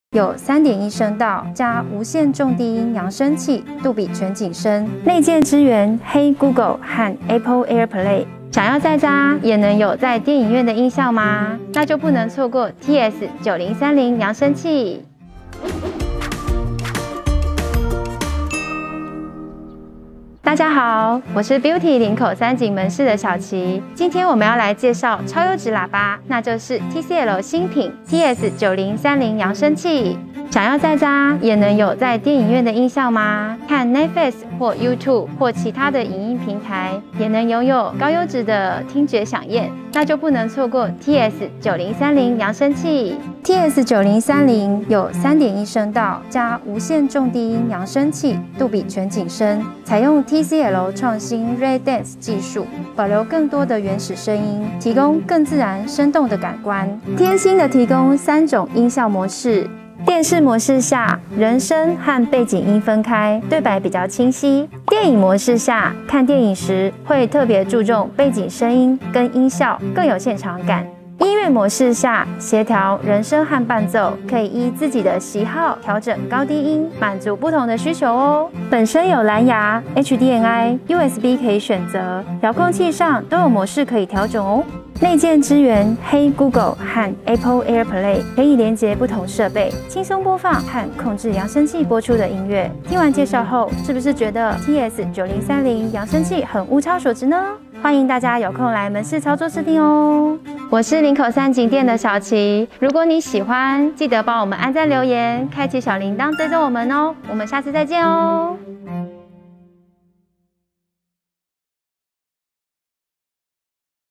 在家也能有電影院般的音效？TCL TS9030 Soundbar feat. 林口三井店 小琪 開箱讓專業的來​​
再怎麼專業，絕對都沒有門市人員來的專業！既然這樣，開箱何不讓專業的來！

今日開箱員：Beutii 林口三井店 小琪
介紹單品：TCL TS9030 Soundbar

在家追劇也能像在電影院一樣過癮
3.1 聲道及無線重低音揚聲器，支援Dollby Atomos杜比全景聲
TCL TS9030 Soundbar 給你絕佳的視聽感受！